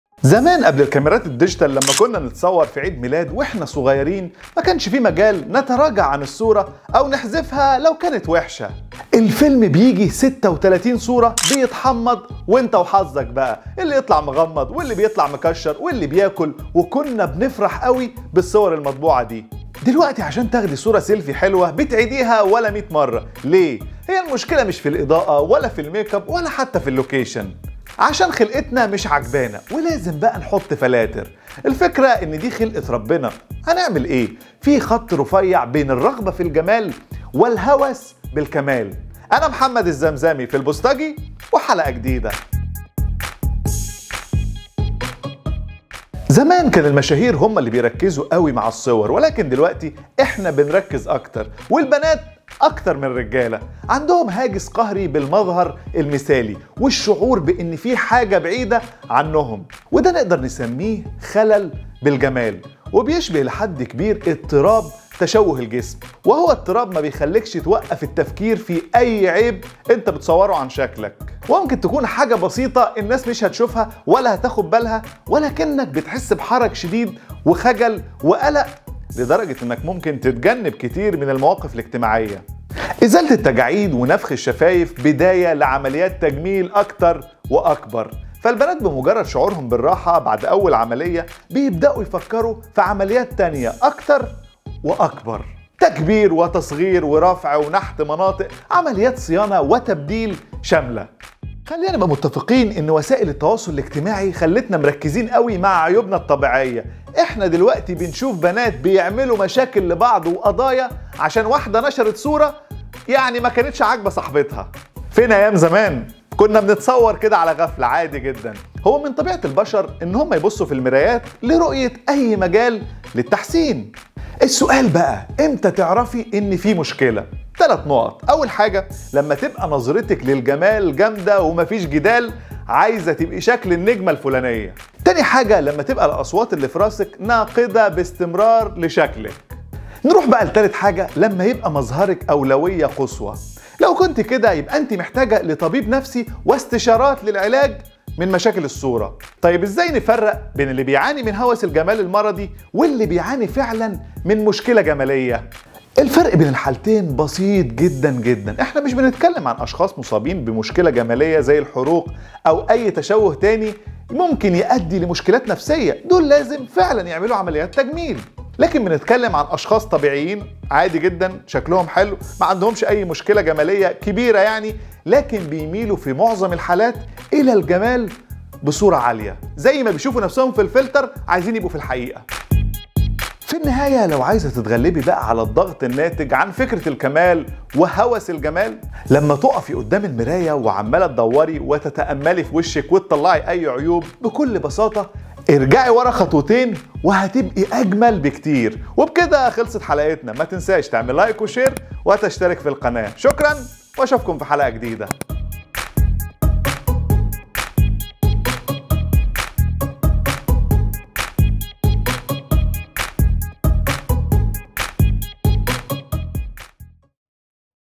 ليه في بنات عندهم هوس بالجمال ؟
زمان قبل الكاميرات الديجيتال  لما كنا نتصور في عيد ميلاد واحنا صغيرين مكانش في مجال نراجع الصورة او نحذفها لو وحشه ، الفيلم بييجي ٣٦ صوره بيتحمض وانت وحظك اللي يطلع مغمض اللي يطلع مكشر ولا بياكل وكنا بنفرح اوي بالصور المطبوعه دي
دلوقتي عشان تاخدي صوره سيلفي حلوة بتعيديها ١٠٠ مره ليه ؟ وهي مش مشكلة اضاءة ولا في الميك اب ولا اللوكيشن صورة عشان خلقتنا مش عاجبانا ولازم نحط فلاتر
 الفكرة ان دي خلقة ربنا هنعمل ايه .. في خيطٌ رفيعٌ بين الرغبة في الجمال والهوس بالكمال